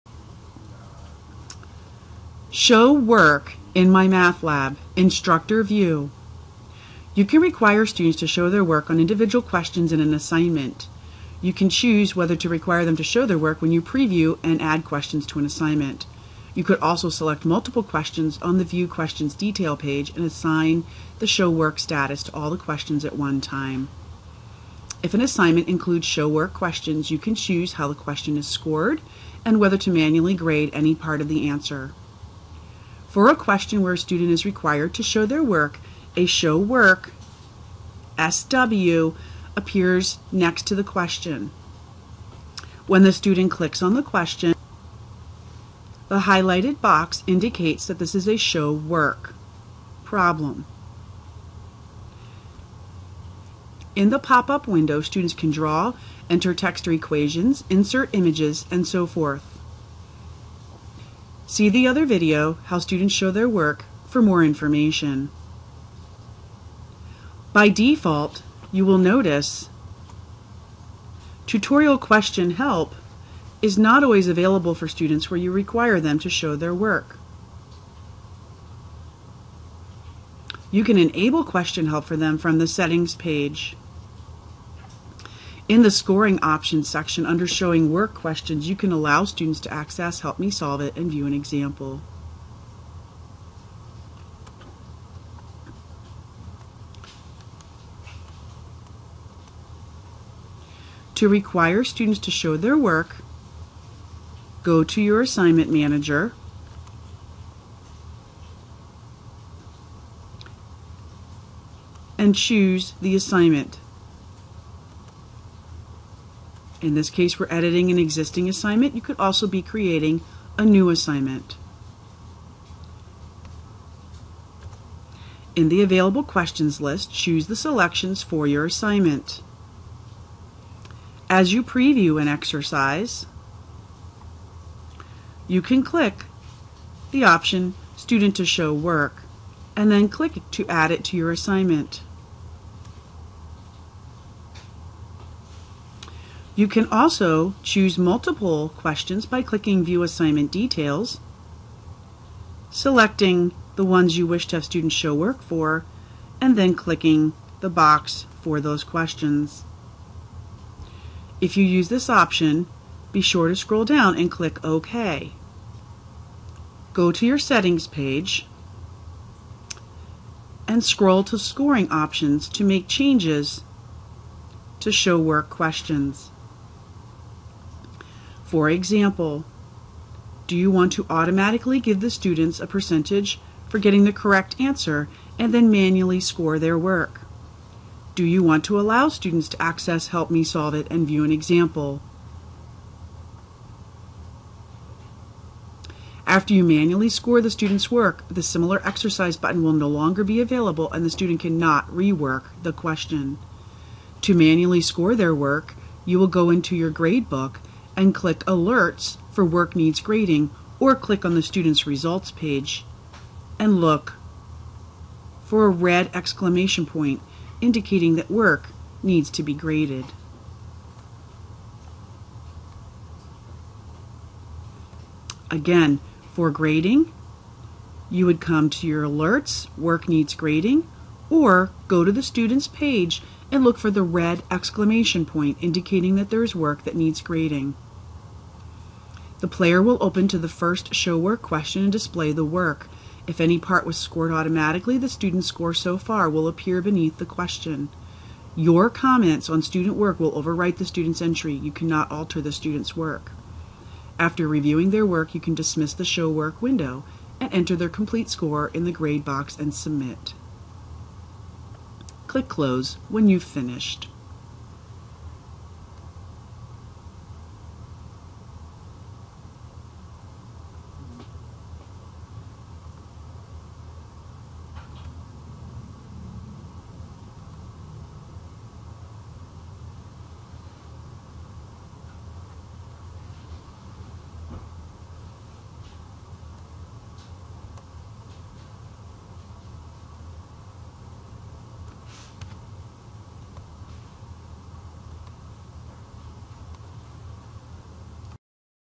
Show Work in MyMathLab Instructor View
This video will assist instructors in requiring students to complete a
Show Work problem. It also shows course settings and where to access student work that needs grading.